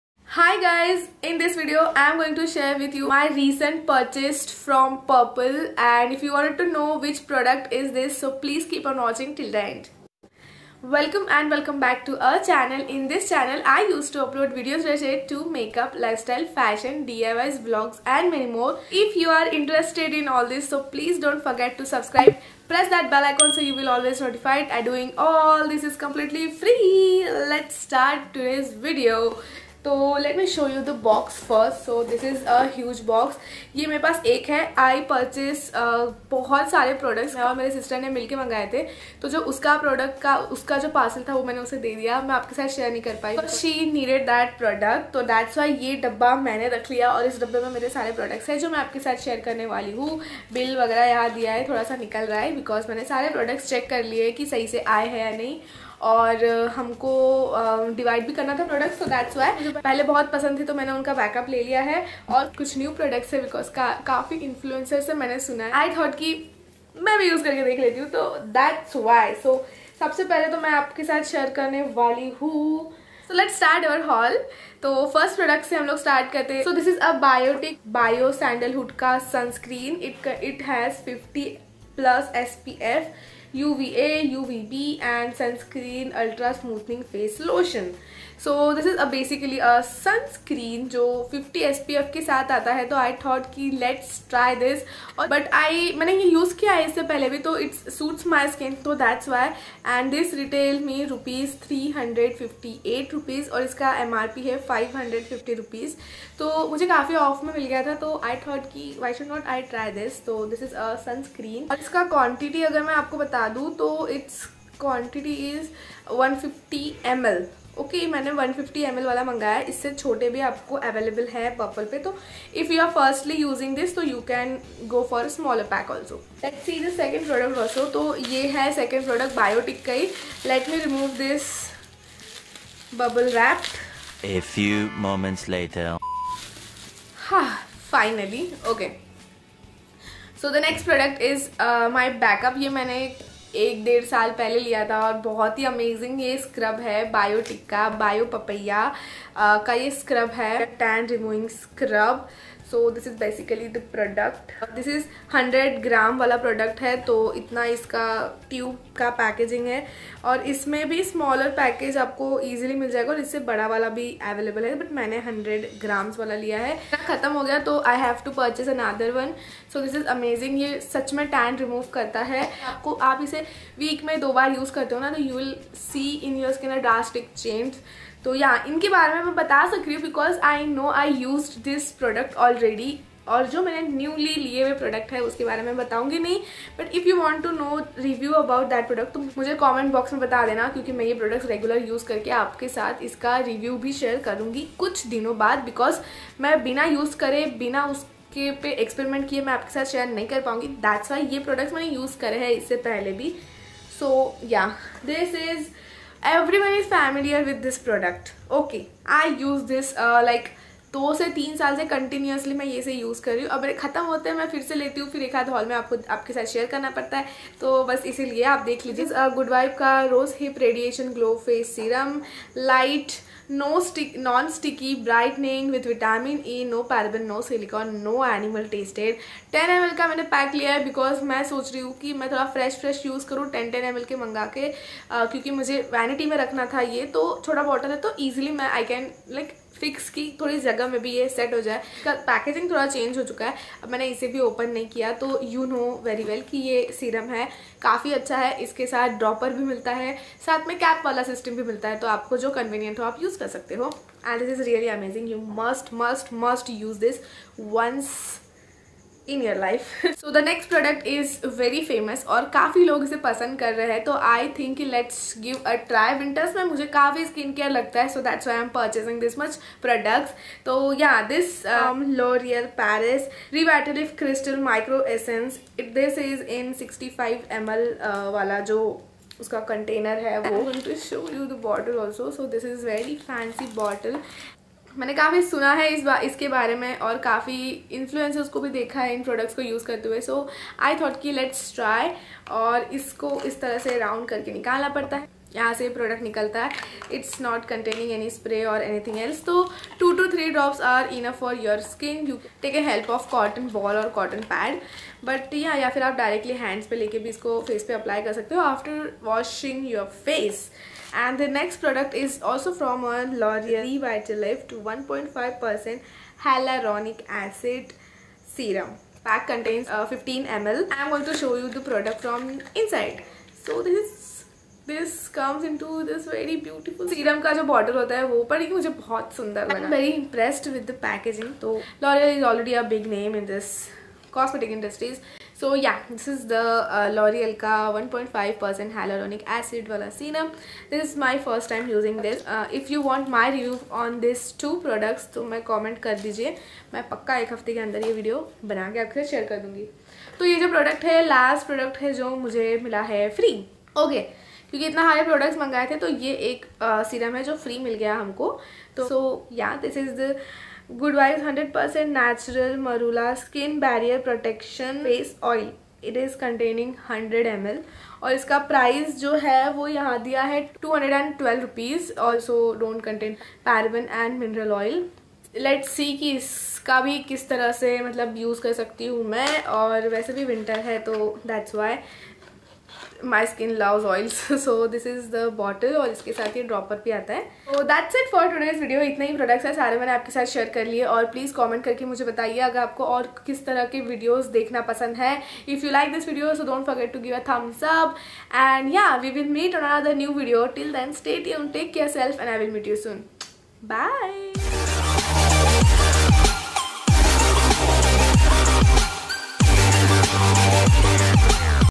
Favorite Purple Haul | Upgrading Skincare | D Stylish
Hey my fav people

here i am sharing with you again purple hall but this time i am upgrading my skin care product and i did some research then i decided to purchase this product so i hope this videos will help you if you want to purchase upgrade you are skin care

I'm on Instagram as @anushreeambatkar31.